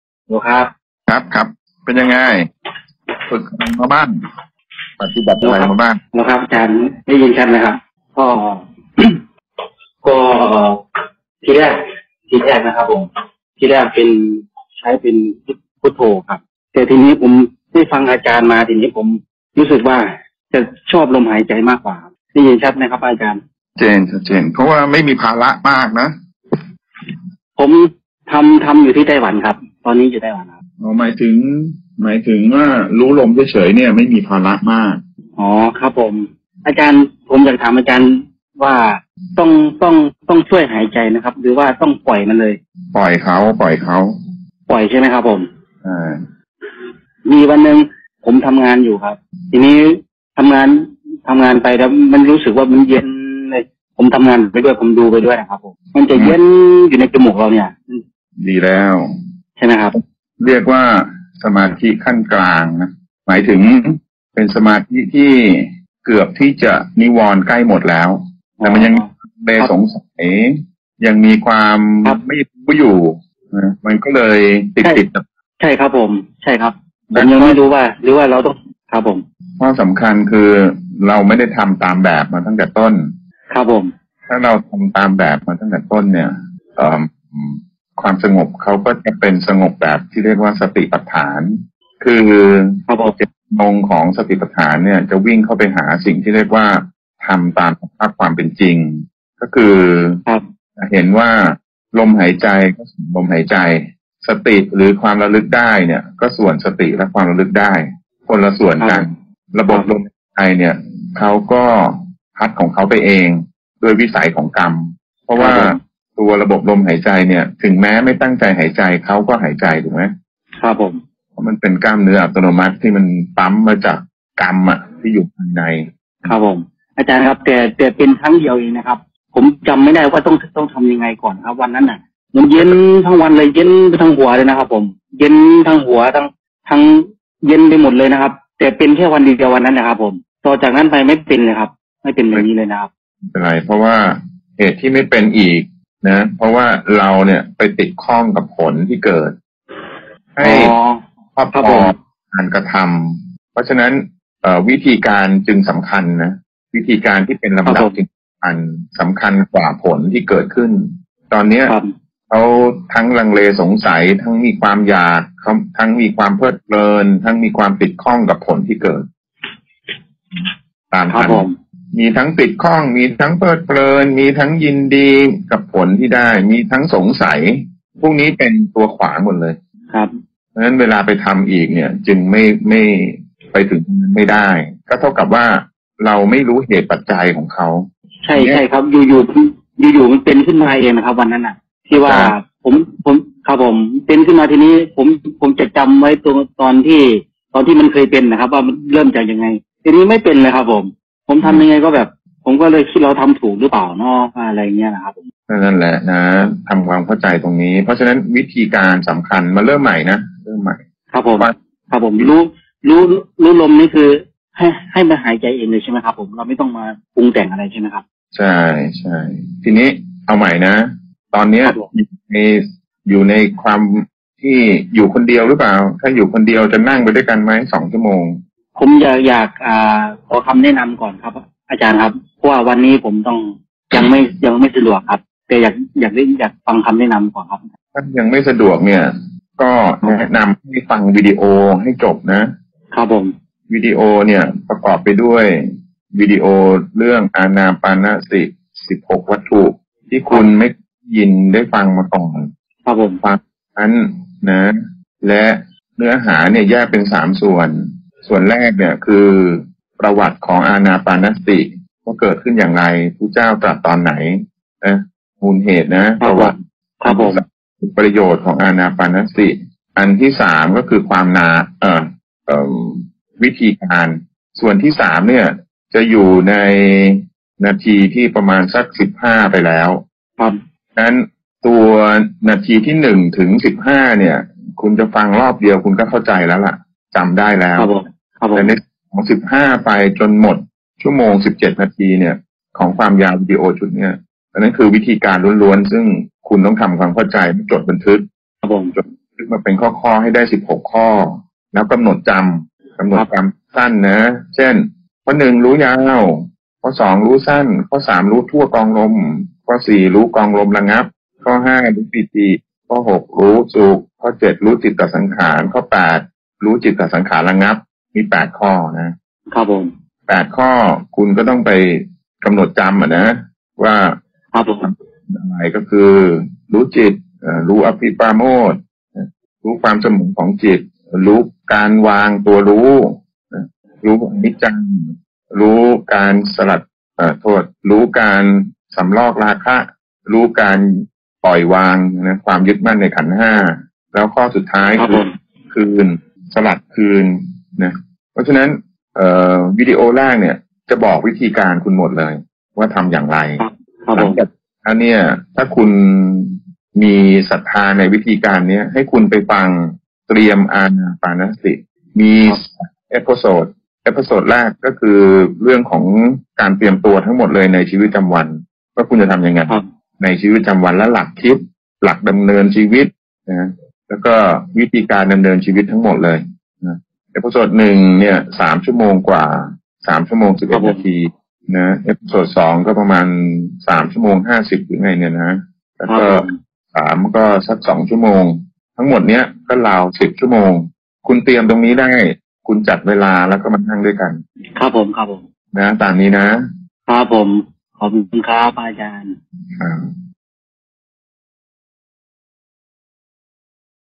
การวางระบบหายใจและความติดข้อง โดย ท่านอาจารย์ Aero.1
📌ท่านใดสนใจเข้าร่วมกลุ่มสนทนาธรรมหรือเข้าร่วมปฏิบัติอานาปานสติ
เชิญกดที่ลิ้งค์นี้ได้เลยค่ะ 👇

📌คลิปที่ได้มาเป็นการบันทึกเสียงผ่านไลน์ ผู้จัดทำเล็งเห็นถึงประโยชน์ จึงทำการเผยแผ่ ต่อสาธารณชน สาธุค่ะ

📌บรรยายธรรม โดย
🙏ท่านอาจารย์ Aero.1 🙏